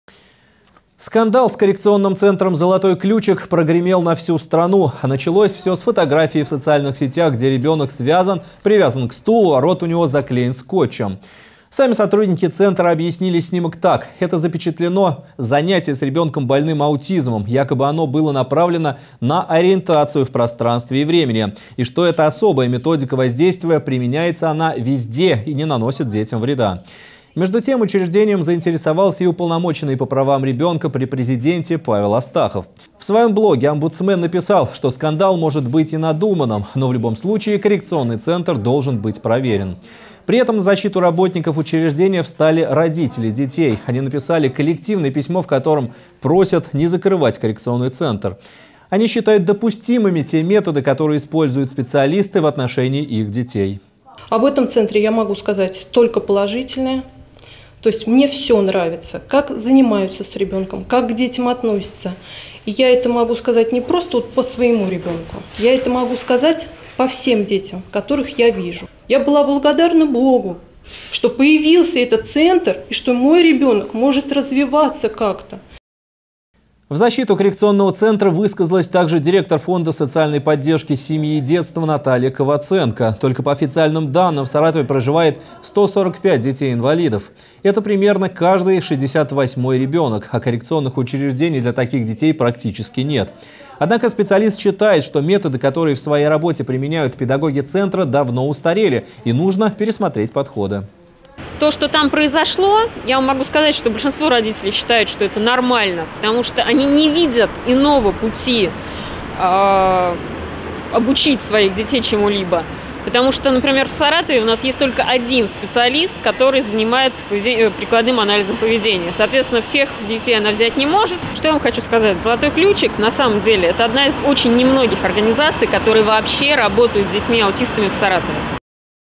Скандал с центром "Золотой ключик" прогремел на всю страну
Сами сотрудники центра объяснили снимок так: это запечатлено занятие с ребенком, больным аутизмом. Подробности узнаете из нашего материала от 16 апреля 2015 года.